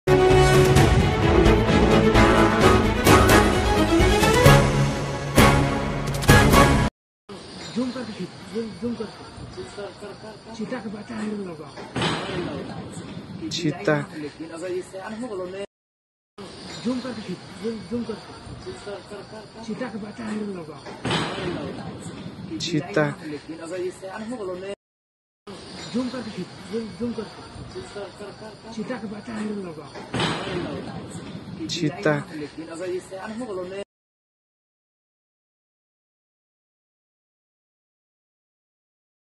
🔴Leopard spotted near Miyapur Metro Station Hyd |7See News Hyd
Following the reports, local police and forest officials rushed to the area to identify the leopard's tracks and assess the situation.